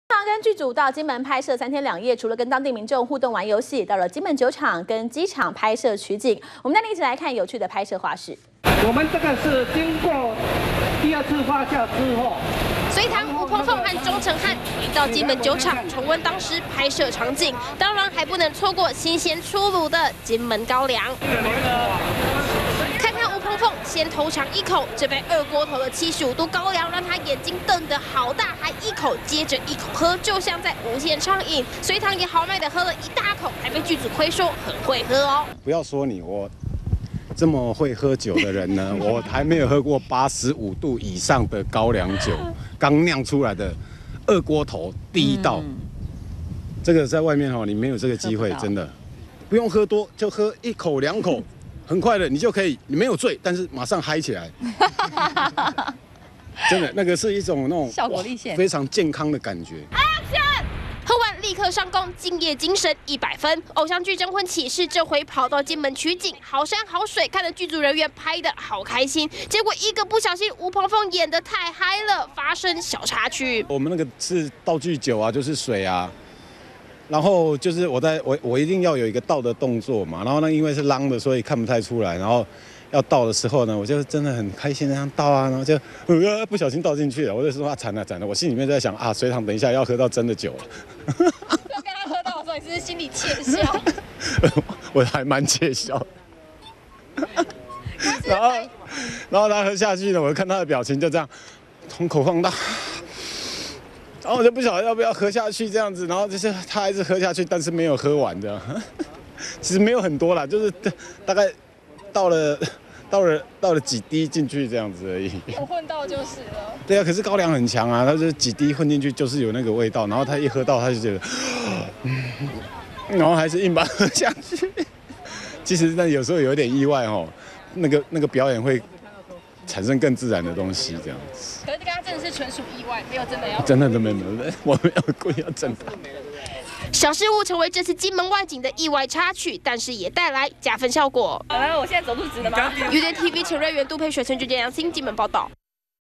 【2014.05.31】隋棠赴金門拍戲 豪飲高粱被虧 -udn tv
隋棠和劇組到金門拍攝三天兩夜，除了和當地民眾互動玩遊戲，也到金門酒廠以及機場拍攝取景，帶您一起來看有趣的拍攝花絮。完整內容 , FB , Twitter , G+